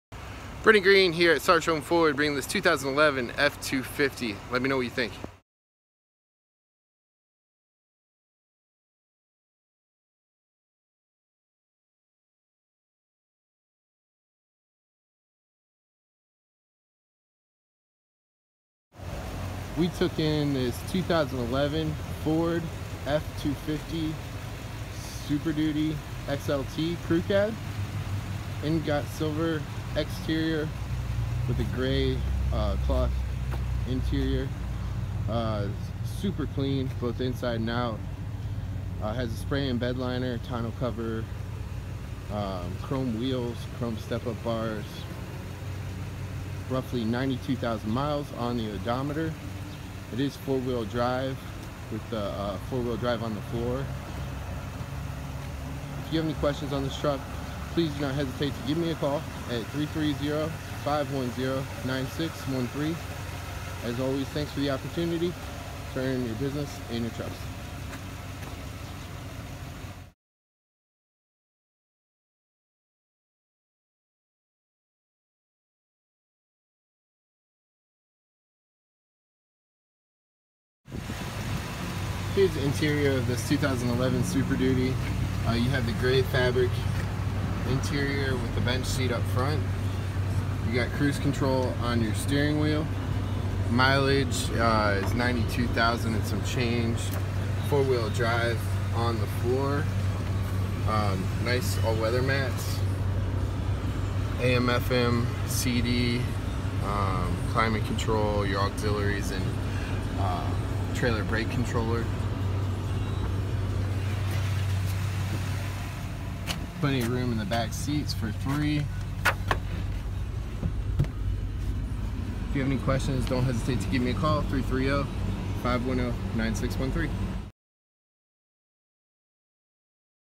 2011 Ford F-250 XLT Crew Cab 4x4 at Sarchione Ford
2011 Ford F-250 XLT Crew Cab 4x4 by Brandon Greene at Sarchione Ford!